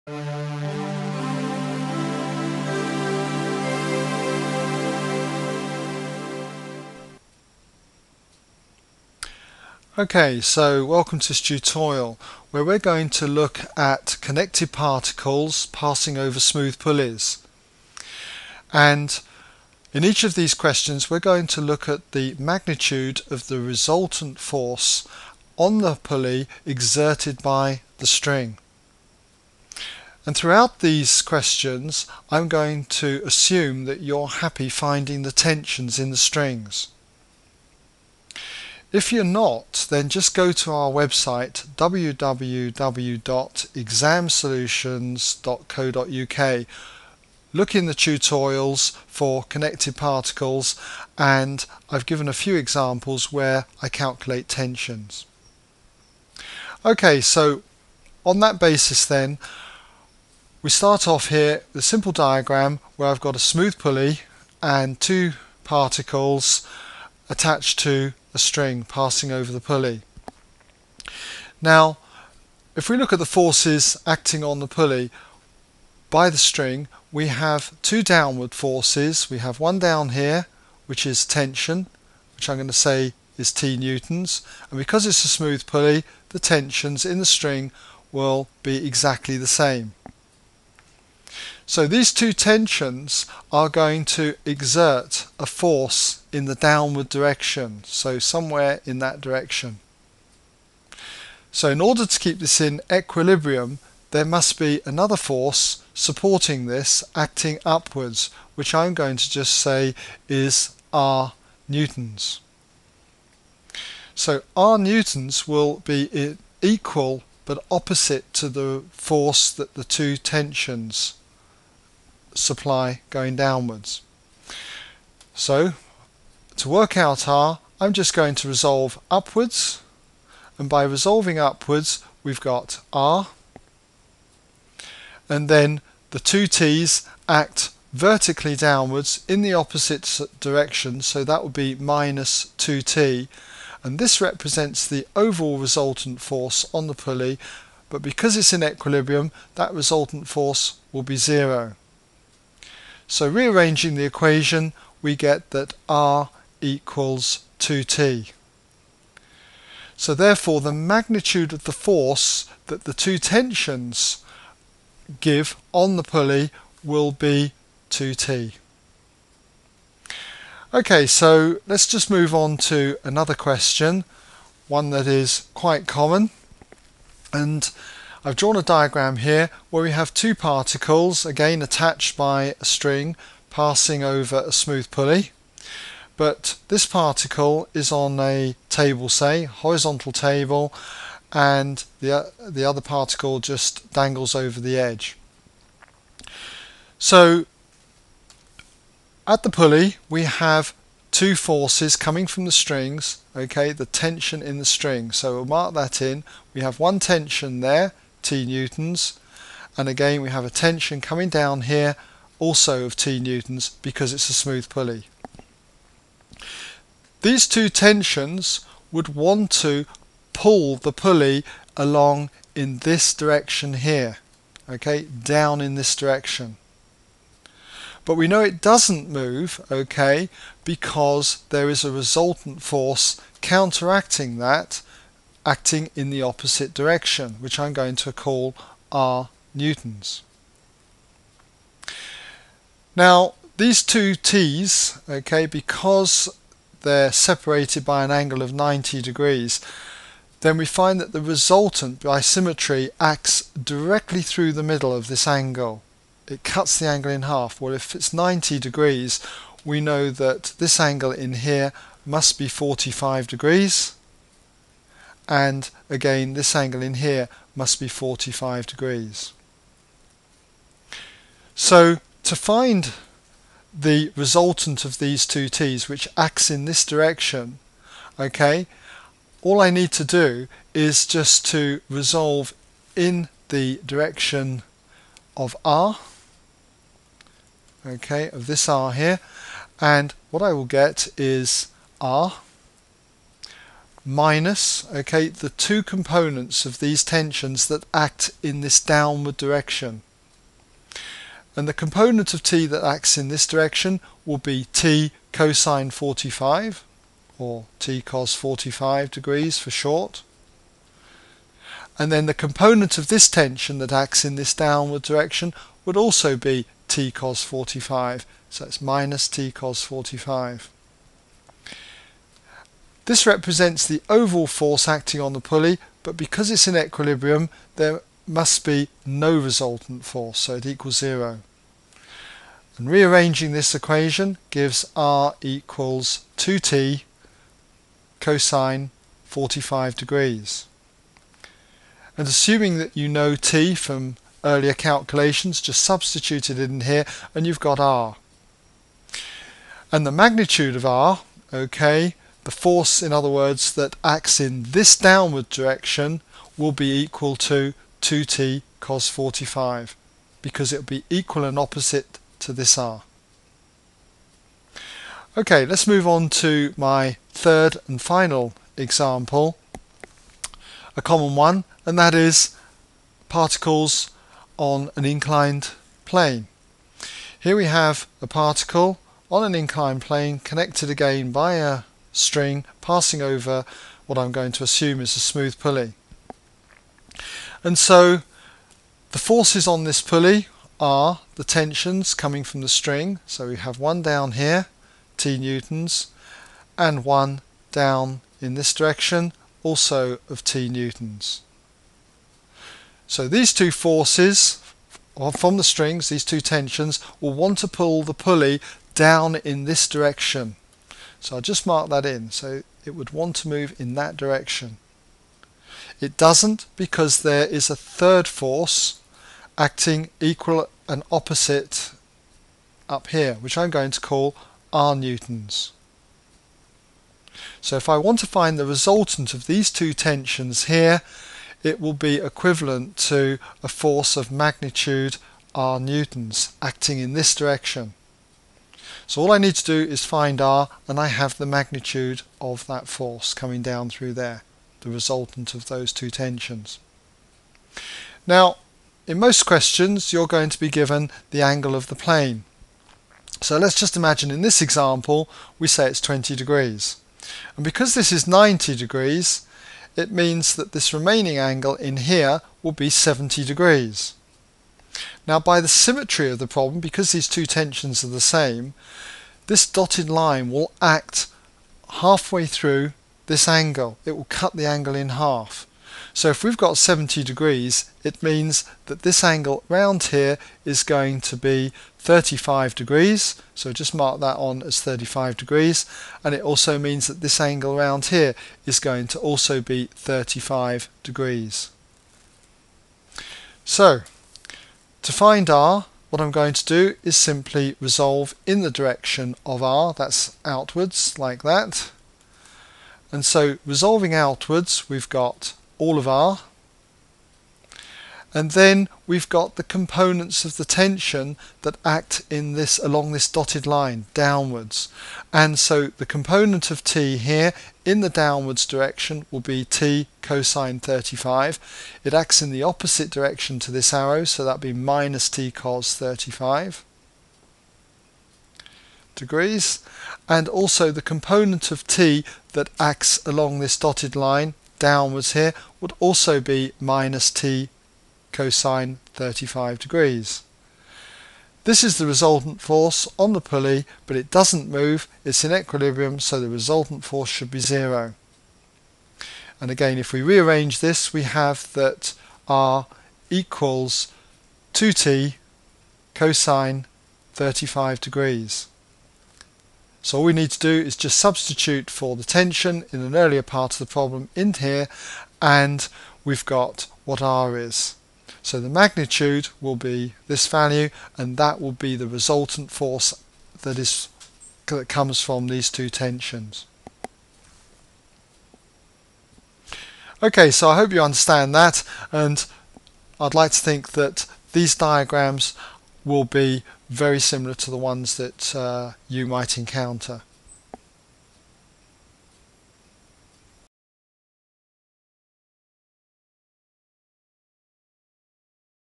Connected particles - Force on pulley by a string : ExamSolutions
Tutorial on connected particles when theres is a force on a pulley by a string.